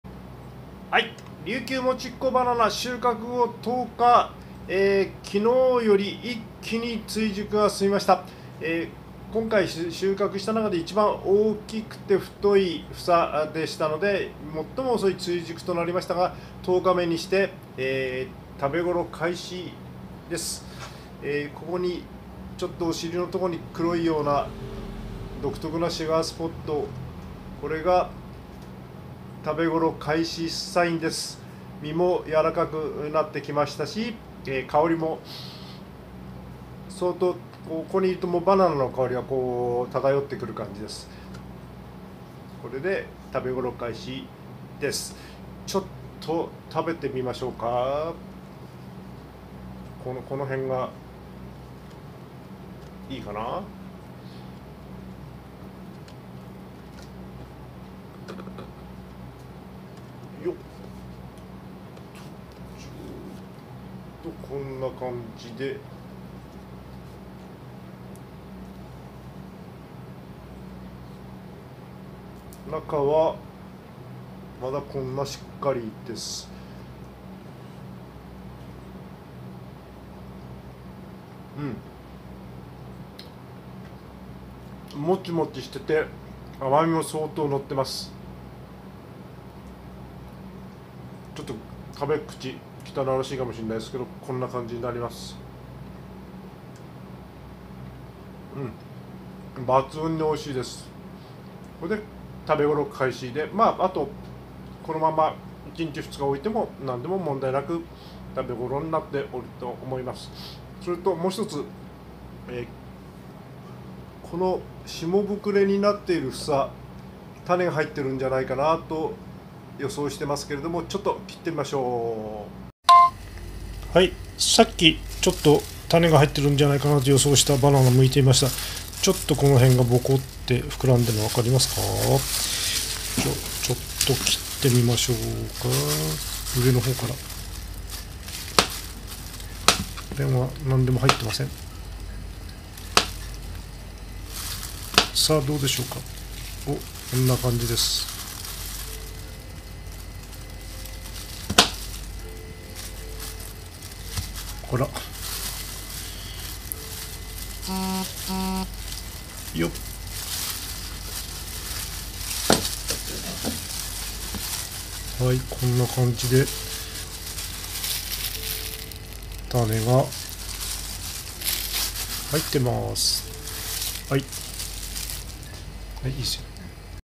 琉球もちっ娘バナナ収穫後10日目 試食＆種入りバナナ 2024年5月編 沖縄物産 真南風 まはえ ( 埼玉県 川越 大正浪漫夢通り） より
琉球もちっ娘バナナ収穫後10日目
食べ頃になりました。うまい！
下ぶくれのようなずんぐりした実は、種を持っていると予想し、切ってみました。
最後の方に切っているところをつないでみました。